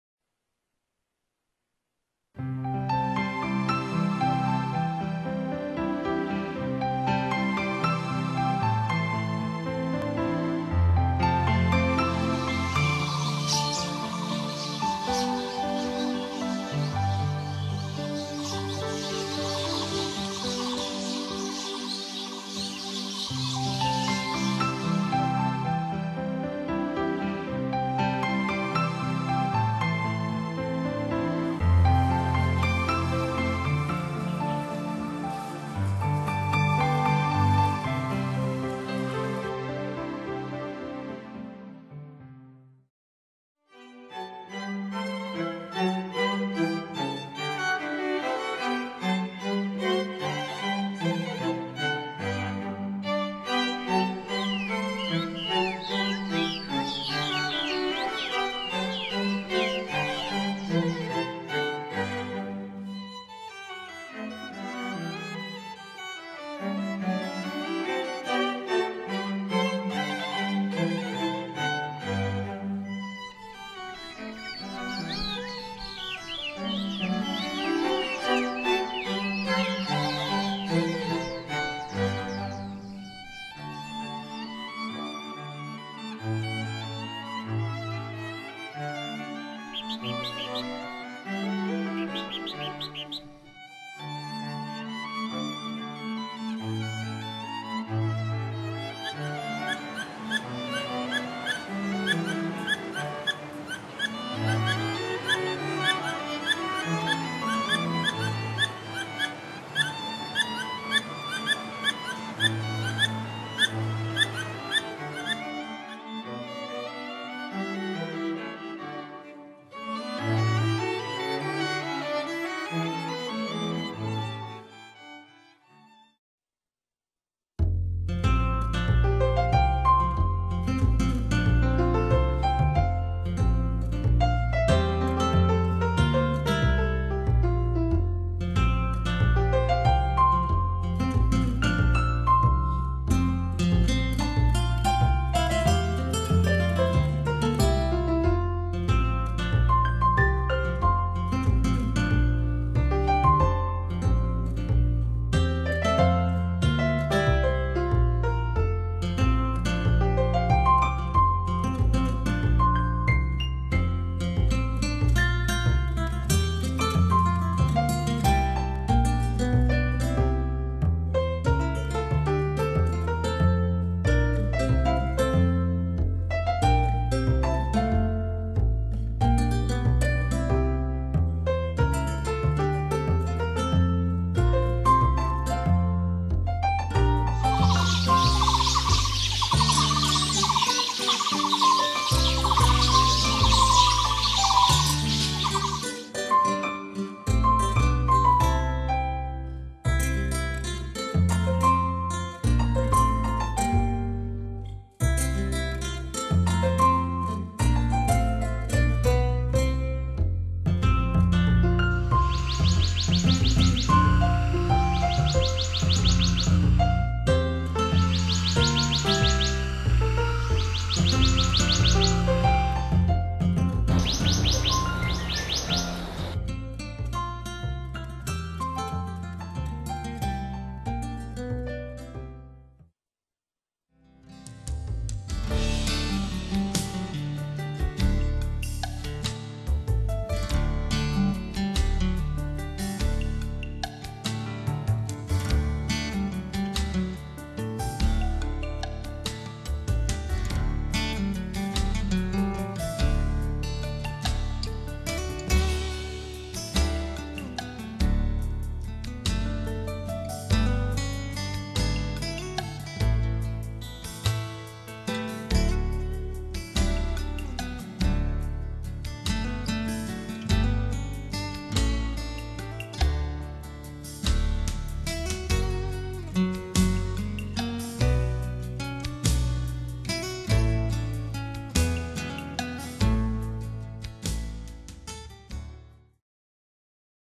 台北植物園自然之美
介紹廣為民眾所熟知的台北植物園內各種生態與活動景緻